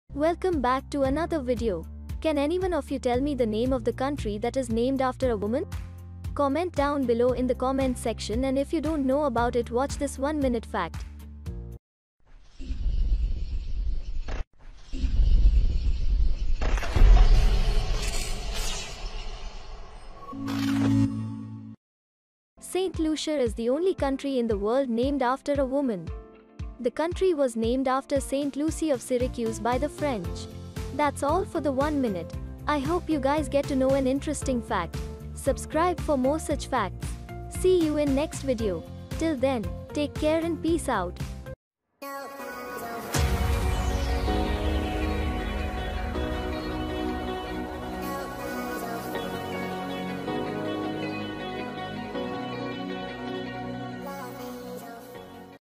The only Country in the World named after a WOMAN | 1M-FACT #16
Saint Lucia is the only country in the world named after a woman.